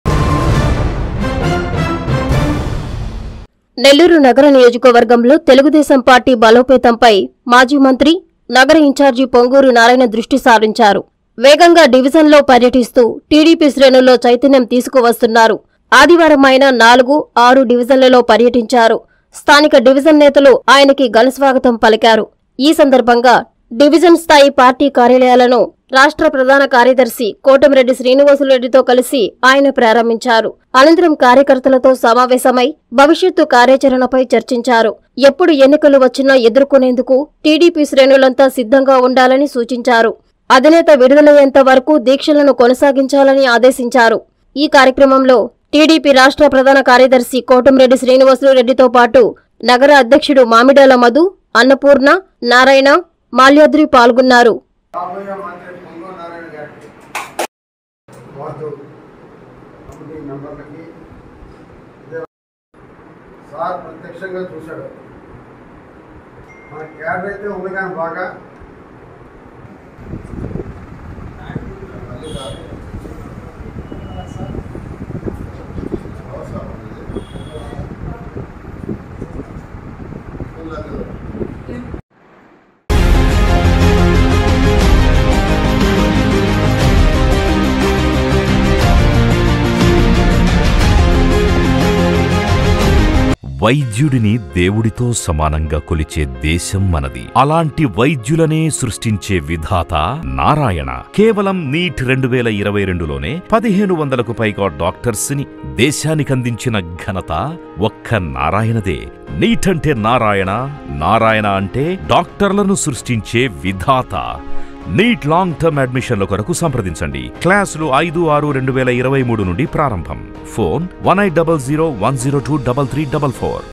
ఎన్నిక‌ల్ని ఎదుర్కొనేందుకు సిద్ధంగా ఉండాలి
- టీడీపీ శ్రేణుల‌కి సూచించిన మాజీ మంత్రి పొంగూరు
- 4,6 డివిజ‌న్ల‌లో ప‌ర్య‌టించిన నారాయ‌ణ